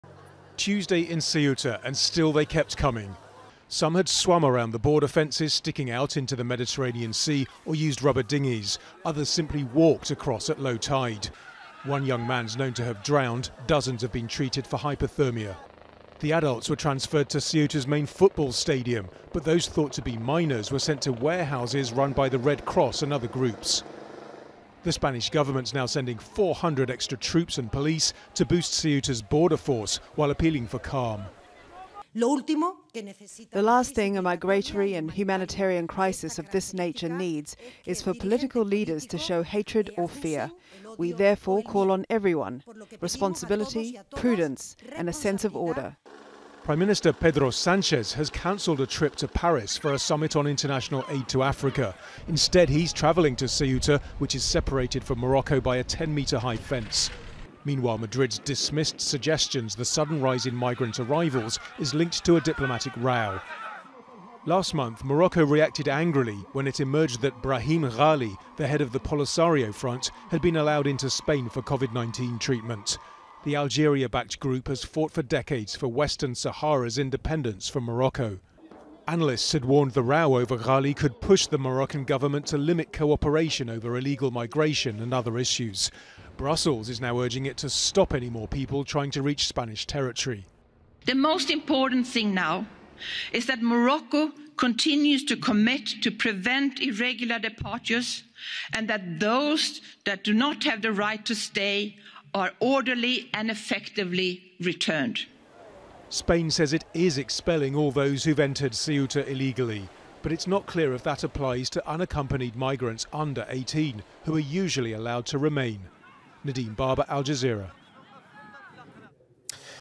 Spain returns people who swam from Morocco, adds troops to border
Spain has sent 2,700 people back to Morocco, out of the more than 6,000 who swam from the North African country into the Spanish enclave of Ceuta over the past two days.
Interior Minister Fernando Grande-Marlaska confirmed the move on Tuesday, adding that Madrid has sent 200 extra police to Ceuta to reinforce the 1,200 officers currently guarding the border with Morocco.

Al Jazeera's Nadim Baba reports.